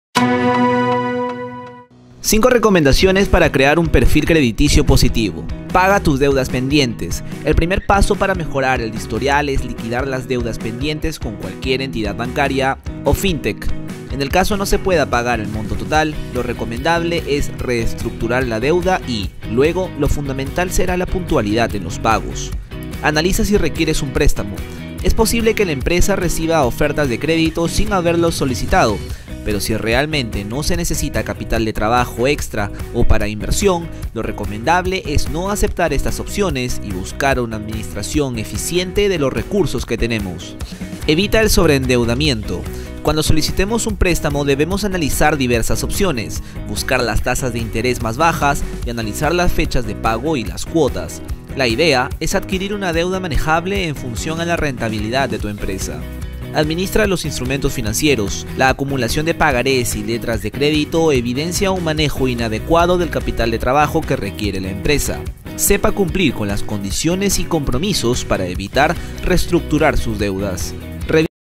Emprendedor: 5 recomendaciones para crear un perfil crediticio positivo
Desarrollar un perfil crediticio positivo nos permite acceder a préstamos de capital de trabajo con entidades bancarias y Fintechs.

Informe y video: Jiordy Arrelucea